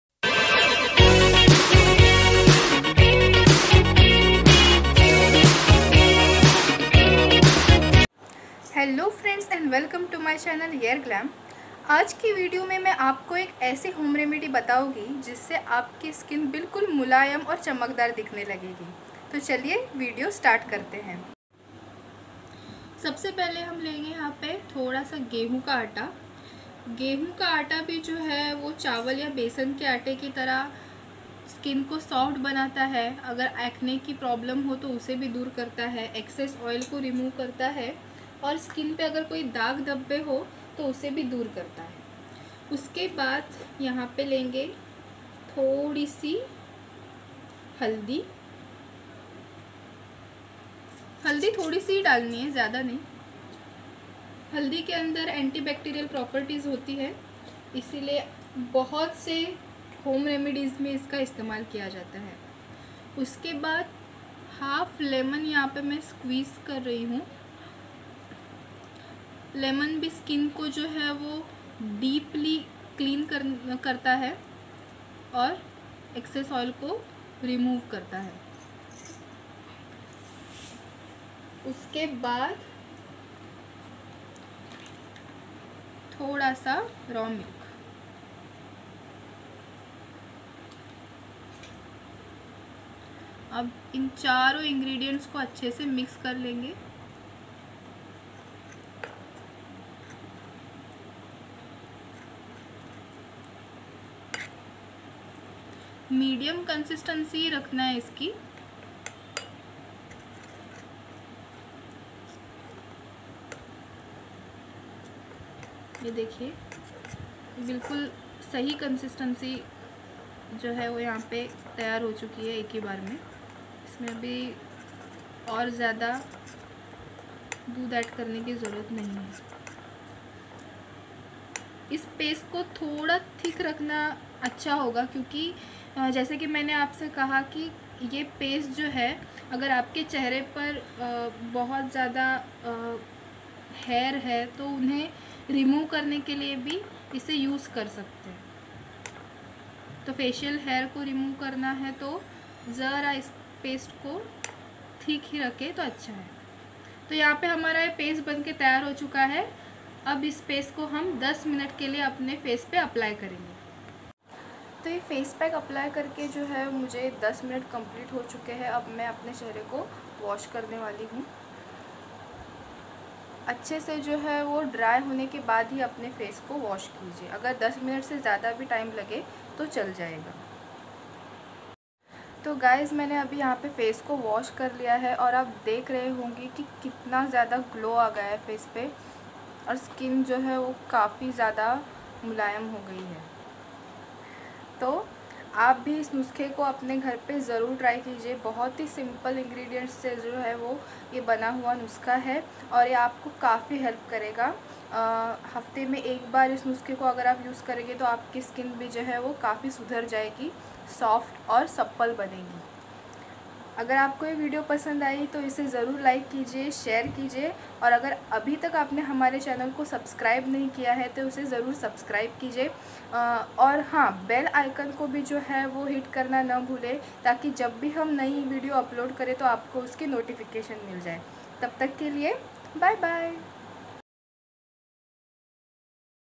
HOME REMEDY FOR SOFT AND SUPPLE SKIN
Hey guys!
In today's video I am gonna show you a home remedy for soft and supple skin 🧖✨